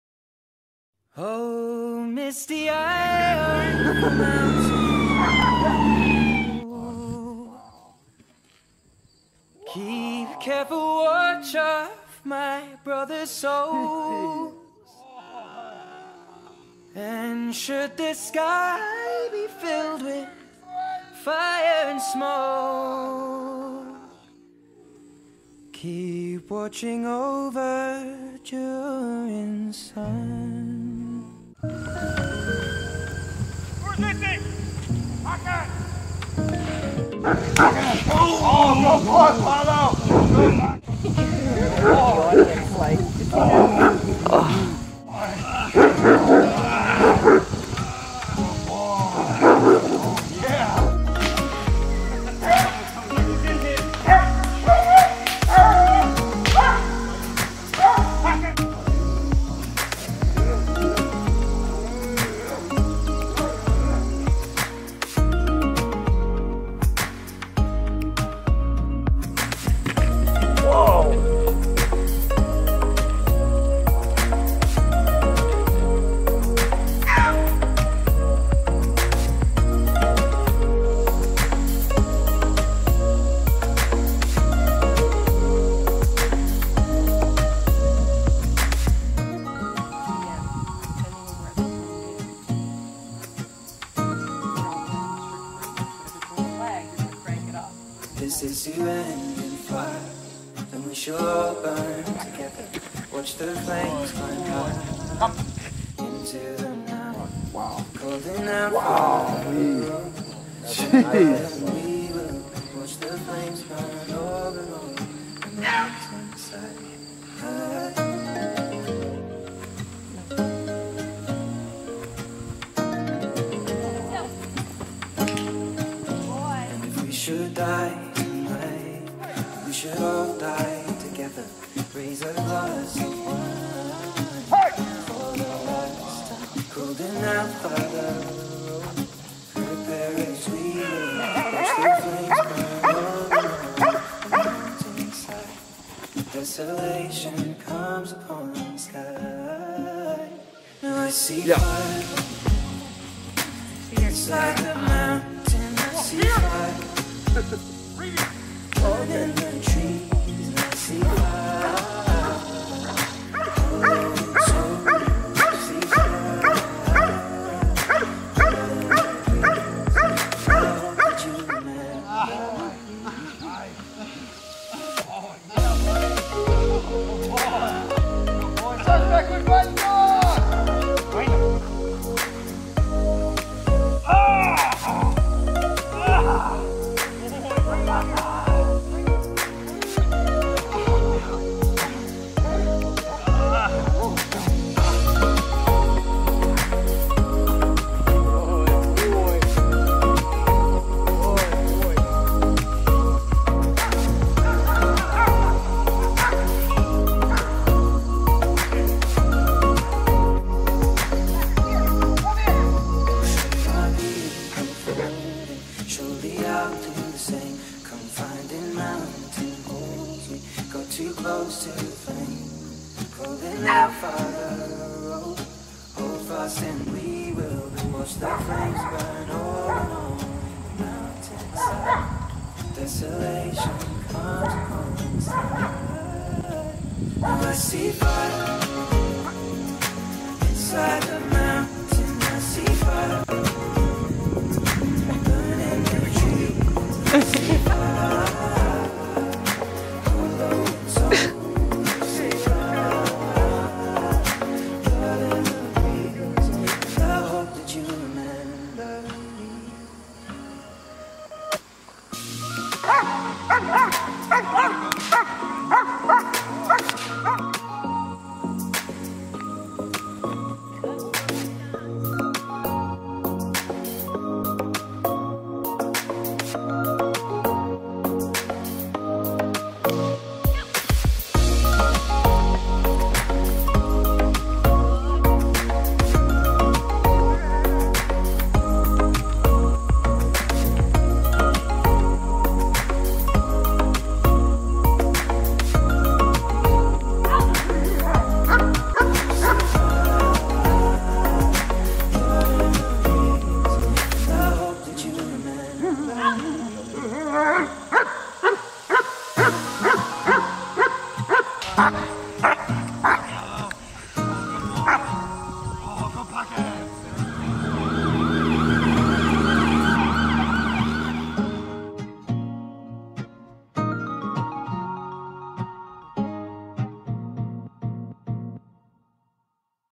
IBSO K9 Training Weekend
Our very own Apollo and Chapo von IBSO (18 Months) Dual Purpose Police K9s came all the way from Michigan City  for our three day seminar.